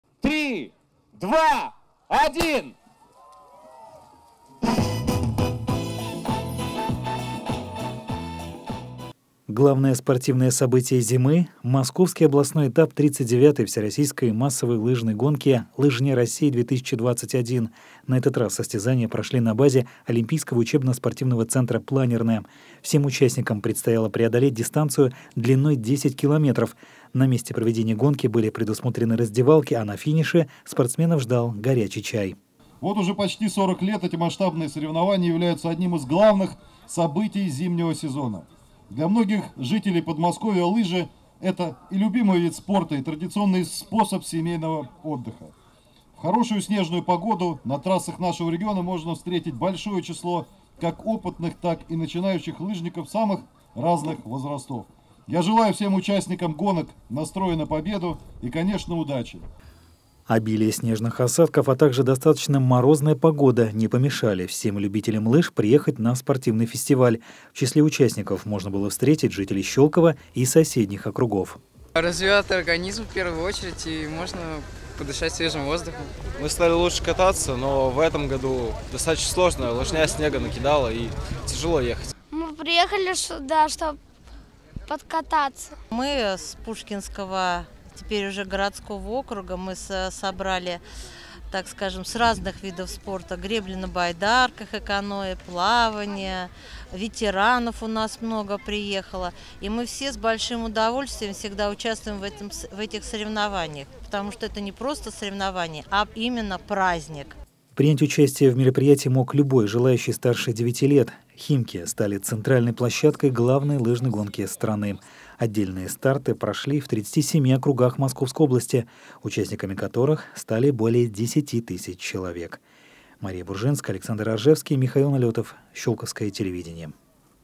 Лыжня России | 5000 человек на старте
Самый масштабный российский старт для профессионалов и любителей лыжного спорта. В Химках на базе учебно-спортивного центра «Планерная» прошла «Лыжня России». Среди 5 тысяч участников свои силы в прохождении дистанции попробовали и щёлковские спортсмены. Как это было, расскажем в нашем сюжете.